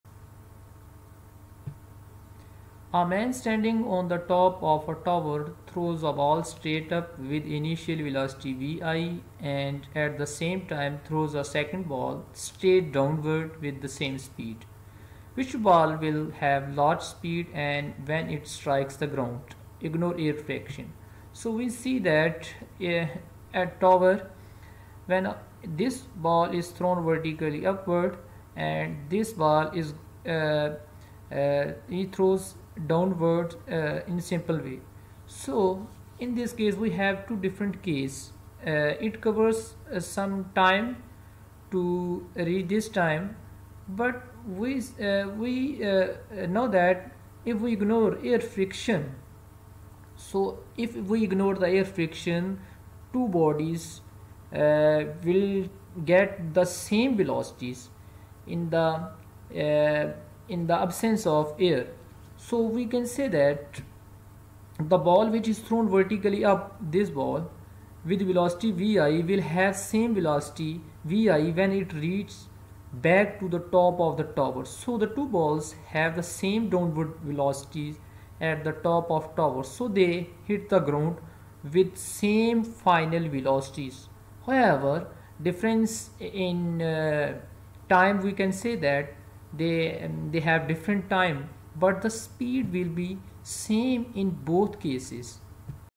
MOTION & FORCE (CH#3)/Exercise Questions 3.5 (1st year)
Comparison of two bodies velocity is being discussed.